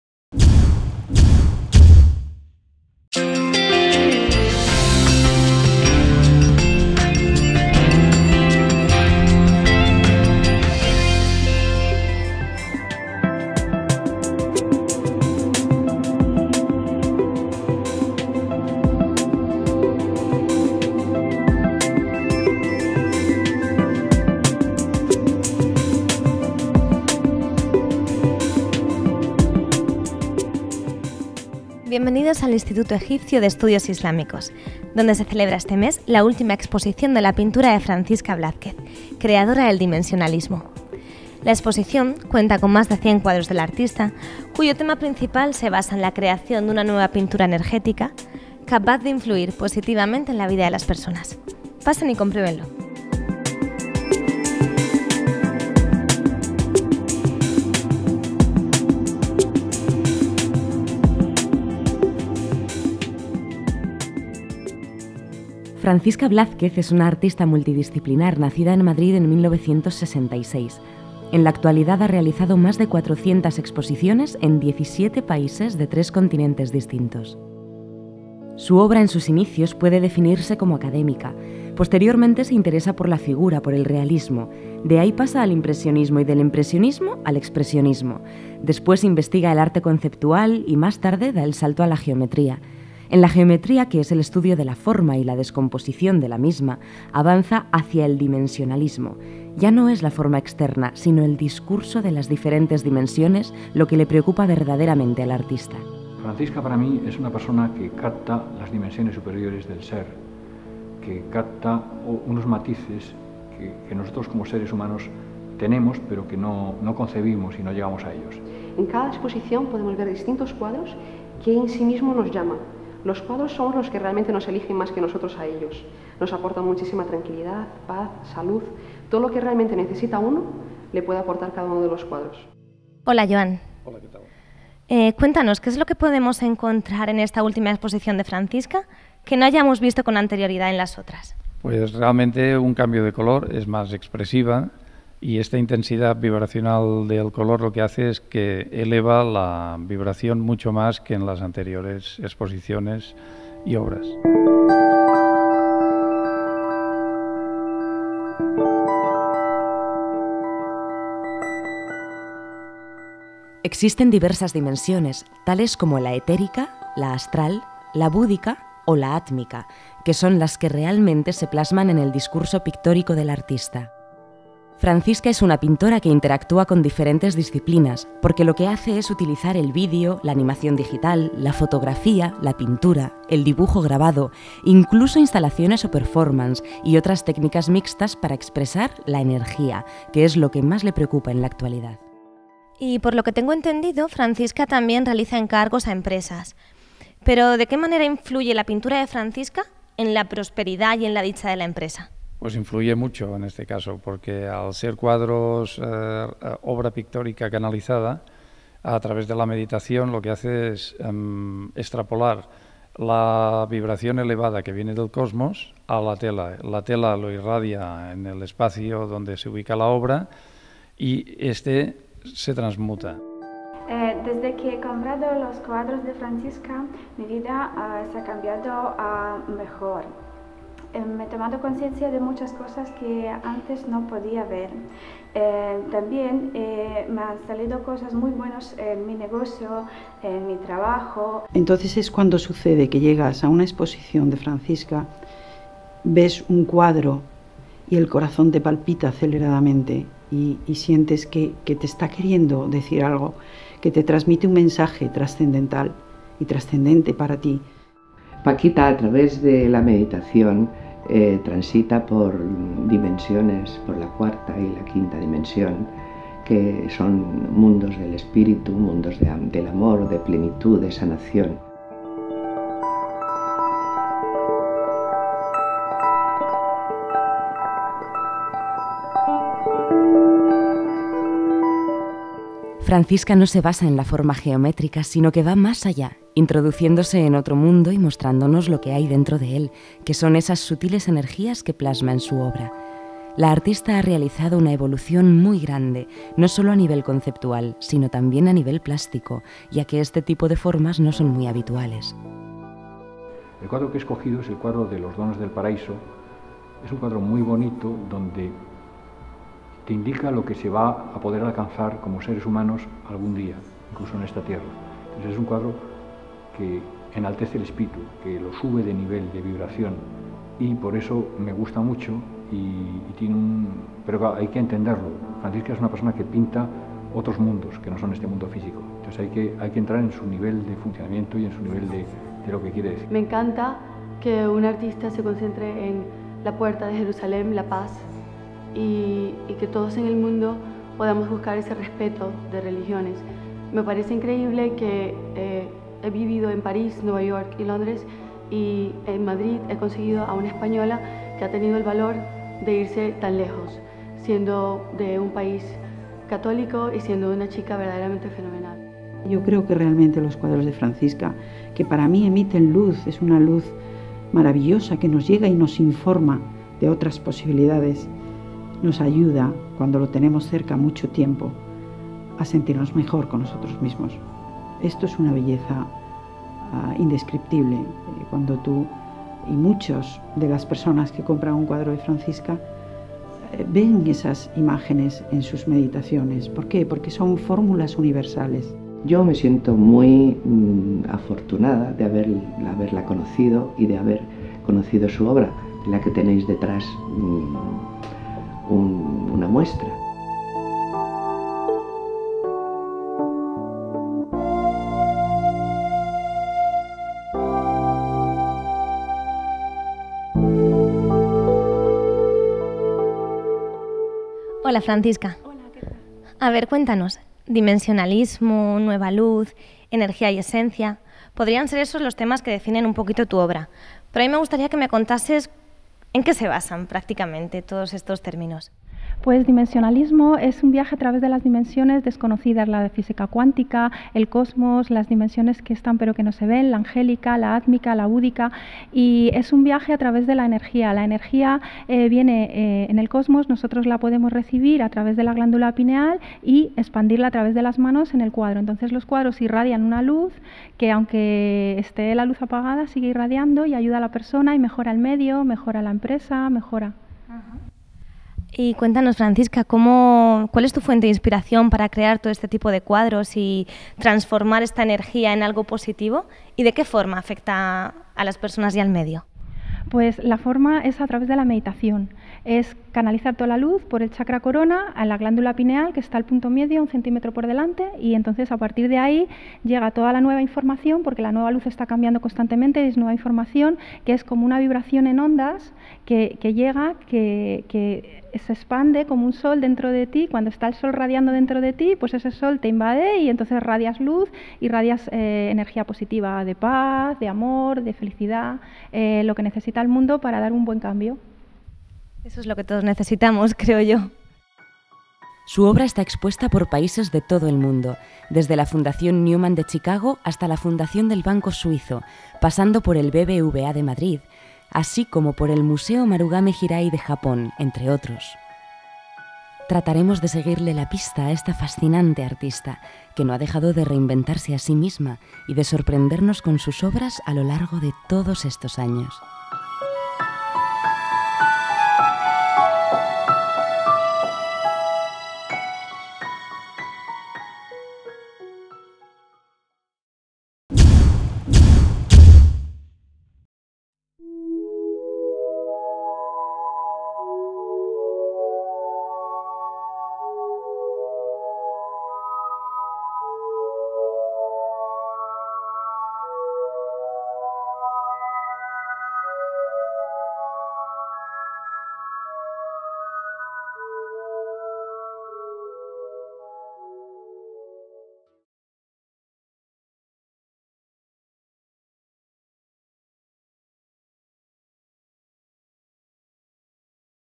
Yupie Films (FRANCISCA BLÁZQUEZ)
Documental sobre la pintora madrileña Francisca Blázquez, creadora del dimensionalismo.
La pieza se centra en la última exposición de su obra en el museo Egipcio de Estudios Islámicos de Madrid, donde numerosos profesionales (psicólogos, doctores,críticos de arte y coleccionistas), reflexionan acerca de la importancia de su obra y de cómo influye positivamente en la vida de la gente, tanto a nivel personal como profesional.